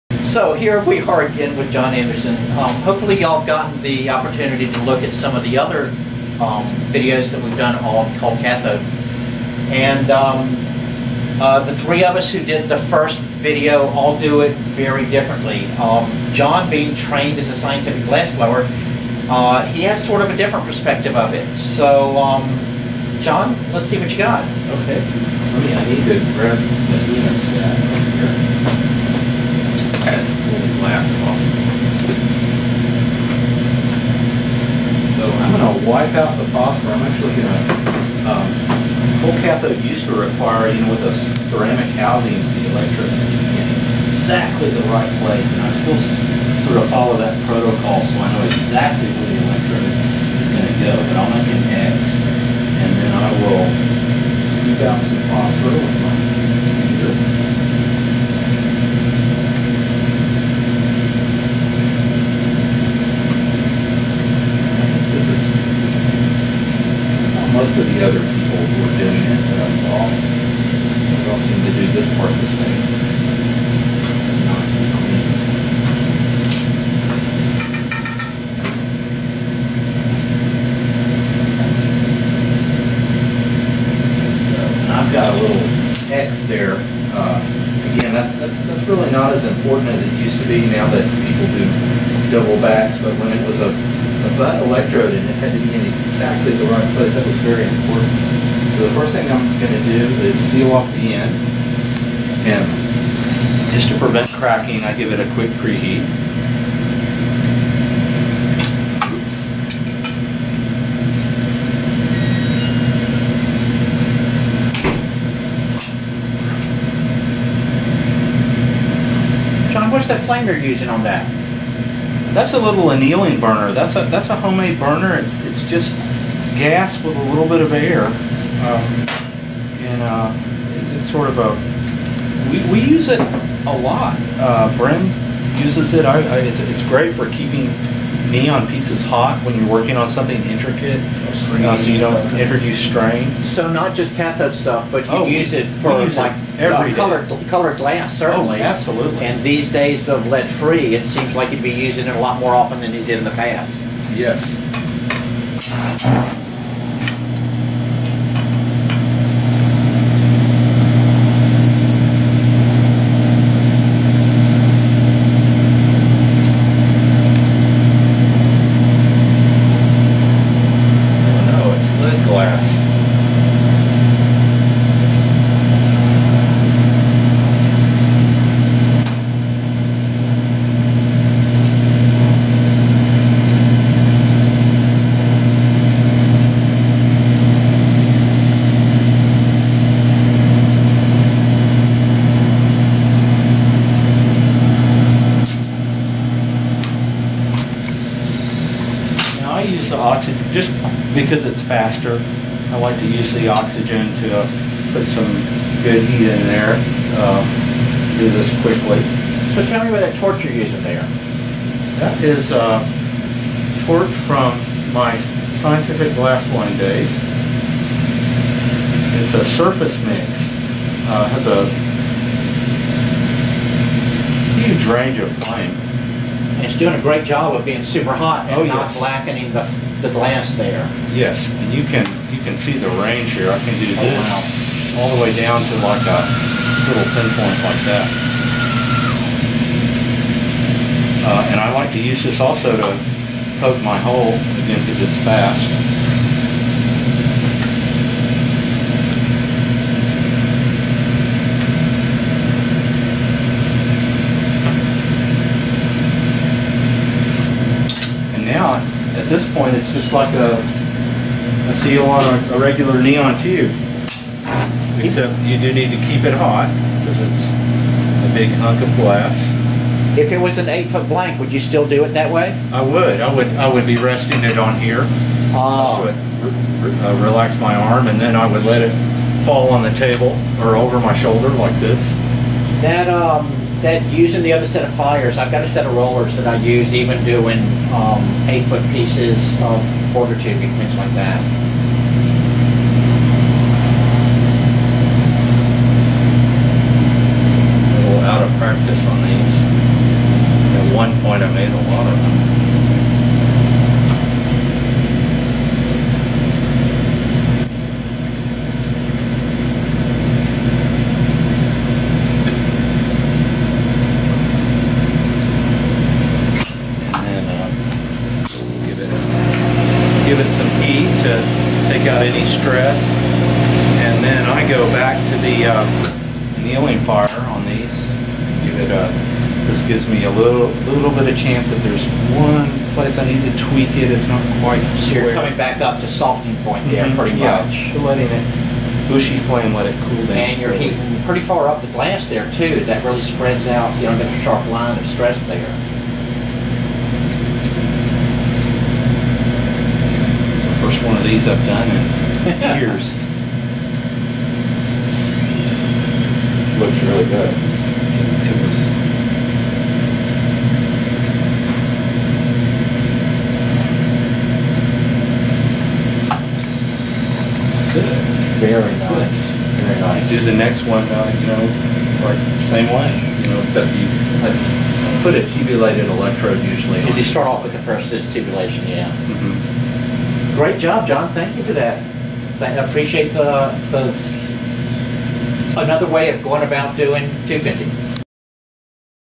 Inside the Neon Shop with John Anderson : Bending Cold Cathode
Watch John Anderson from Mega Volt show Morgan Crook, Ventex’s General Manager, his technique for bending cold cathode. To see more cold cathode videos scroll down on our YouTube to see more experts share their craftsmanship.